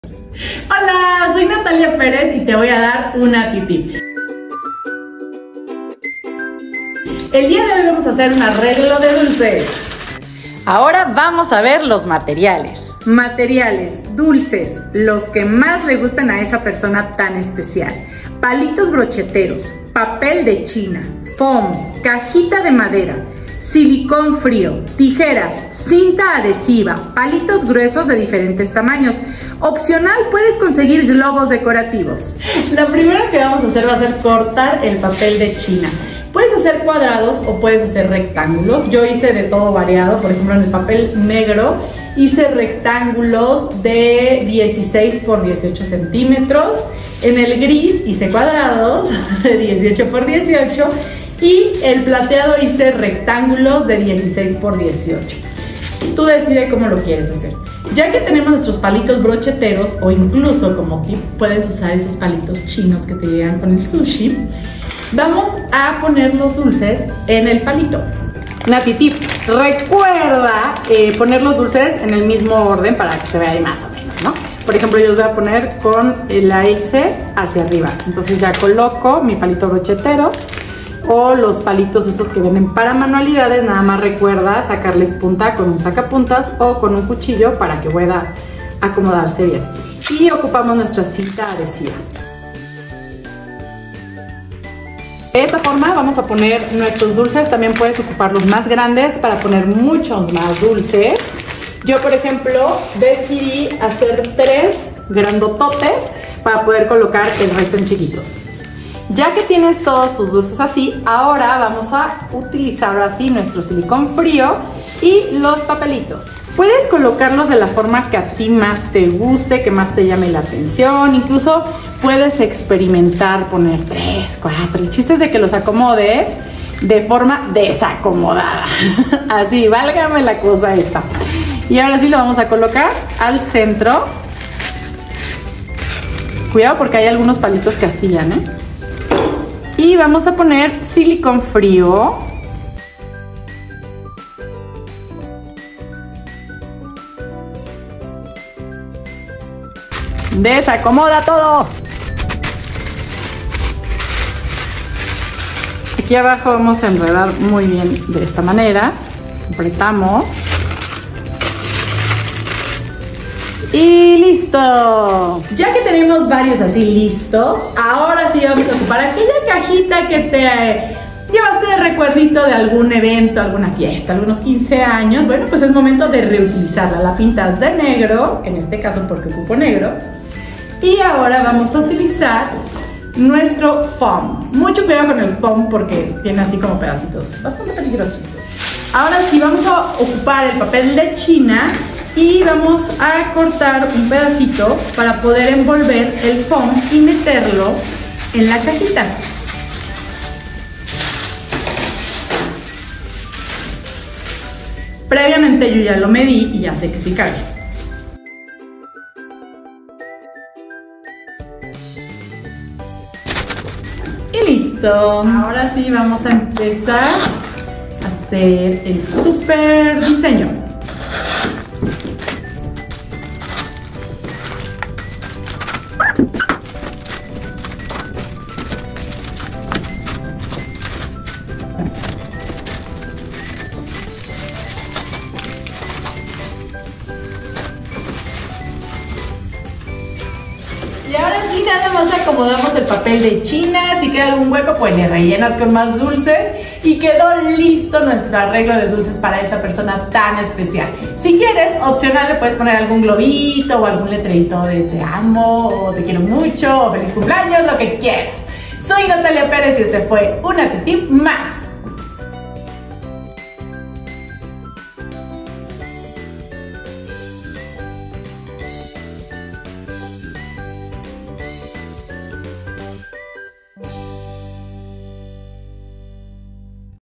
ARREGLO DE DULCES
Este arreglo de dulces es un extraordinario regalo para el amor de tu vida, para algún super amig@ a quien quieres muchísimo, también puede ser un muy buen regalo para una graduación, cumpleaños, día de la madre, del padre, día del maestr@, del niñ@etc.

TAMBIÉN PUEDES EMPRENDER TU NEGOCIO DE ARREGLOS DE DULCES 🍬 🍭 🍫 💰 💴 💵 💰